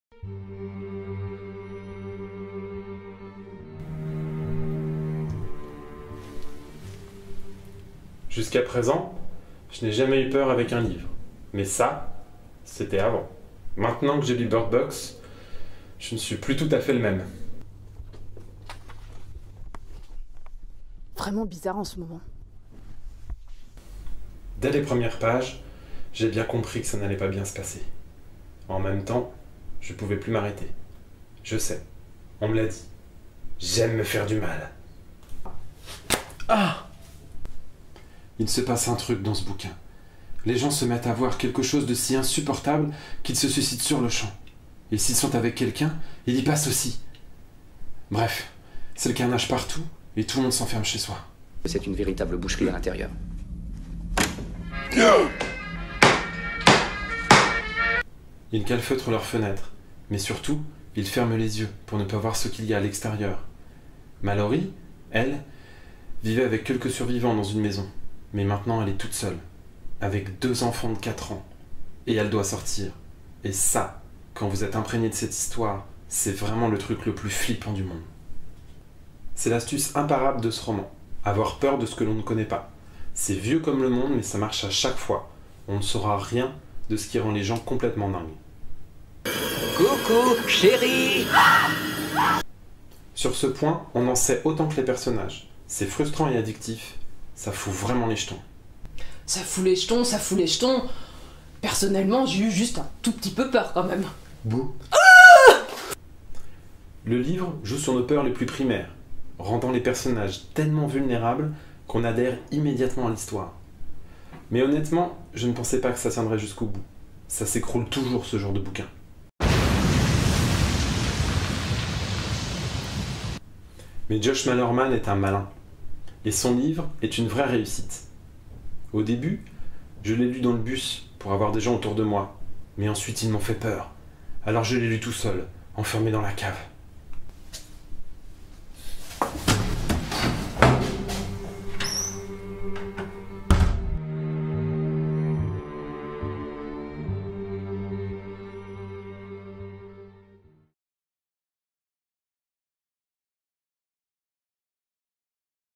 01 - Bird box, Josh Malerman
Des coups de ♥, des bibliothécaires, une caméra...
Voici les booktubes de la médiathèque !!

Pour notre première vidéo, venez frissonner avec Bird Box, de Josh Malerman !